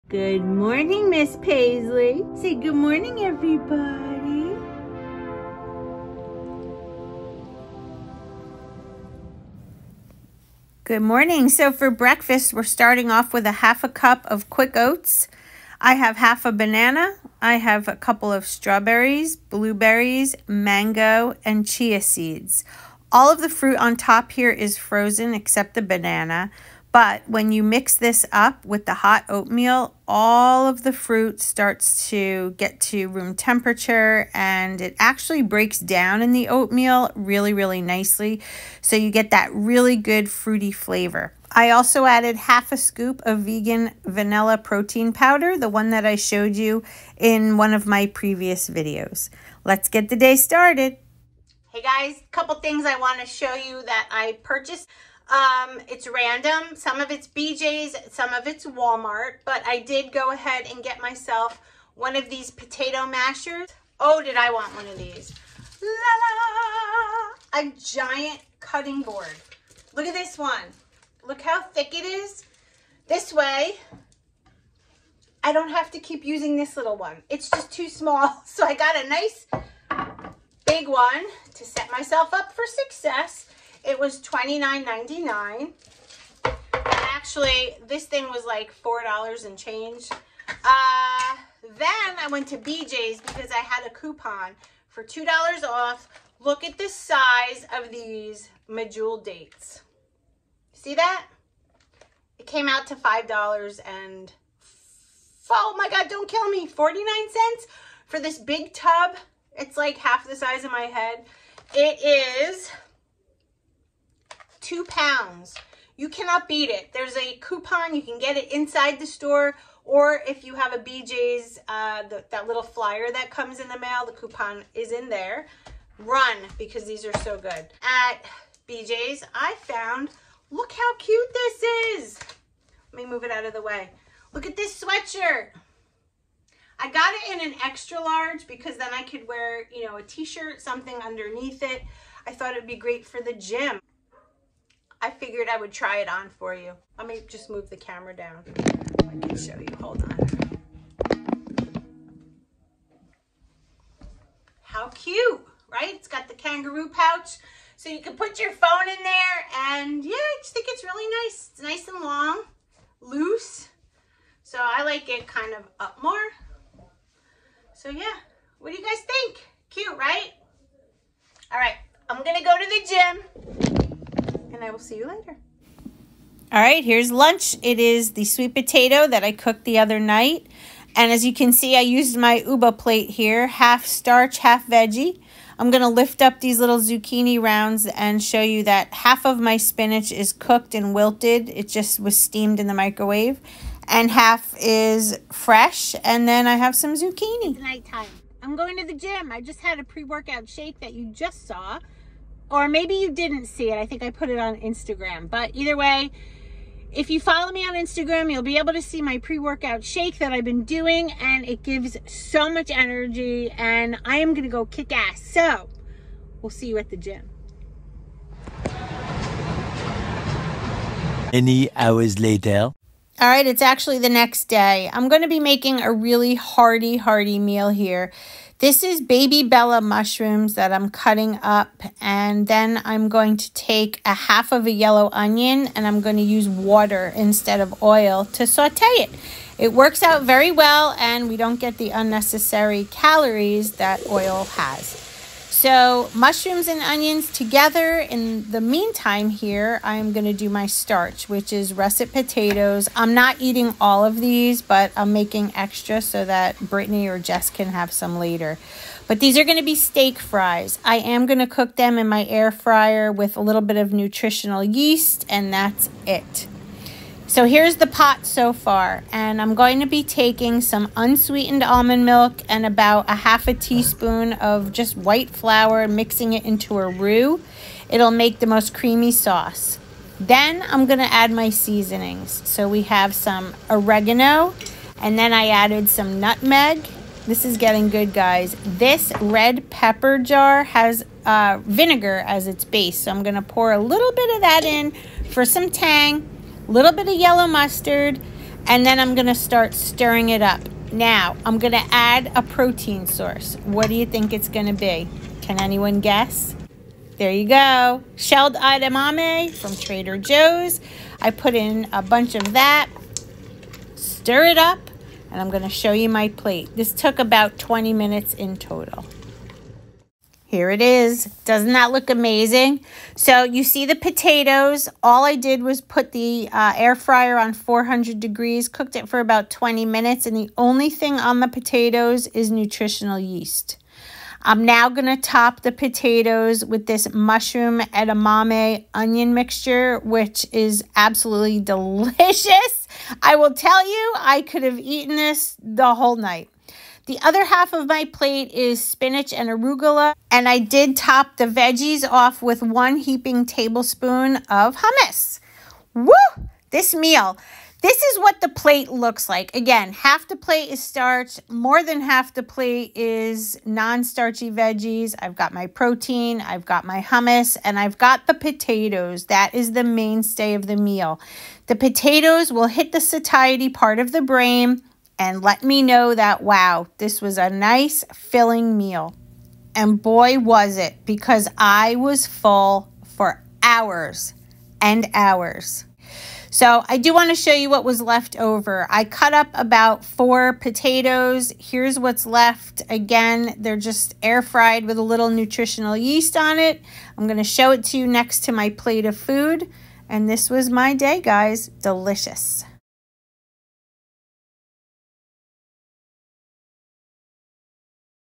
MUSHROOM STROGANOFF/DISCO FRIES// PLANT BASED MEALS FOR MAXIMUM WEIGHT LOSS
⭐️FOLLOW ME⭐️

FACEBOOK/ Keeping it Healthy with Feliciannurse